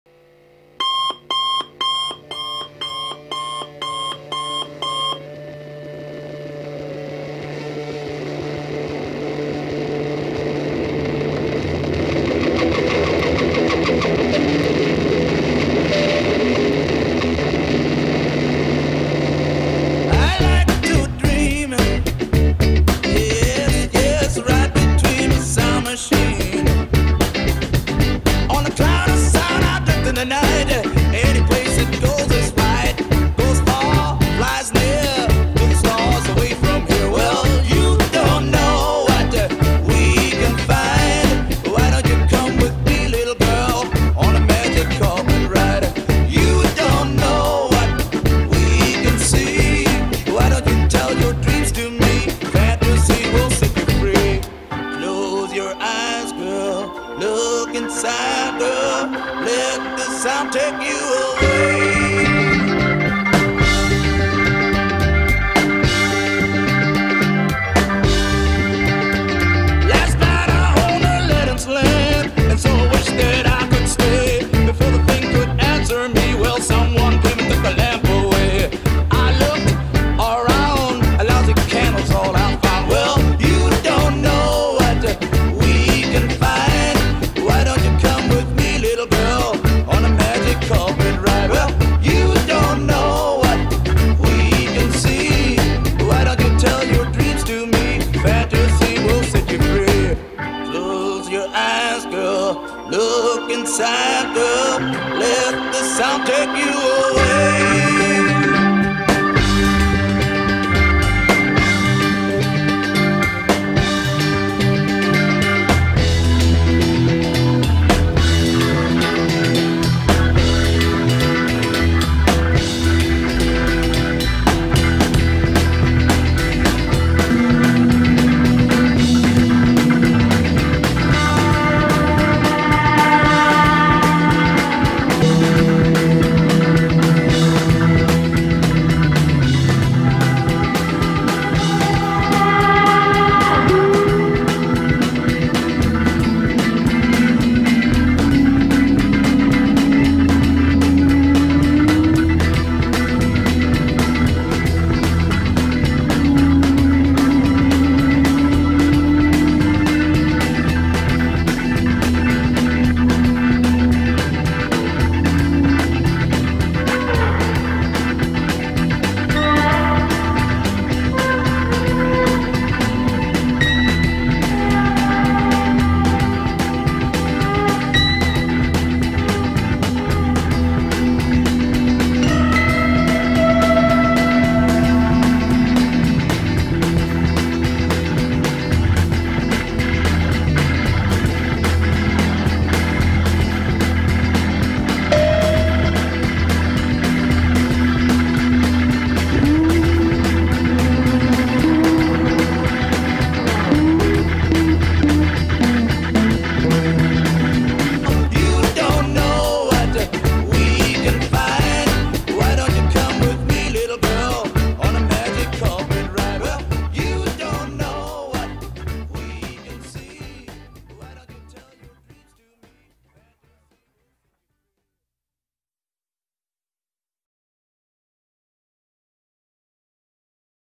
from the 60's
Steppenwolf, a 310, and 2 rockers from the 60's. almost bellbottom psychedelic.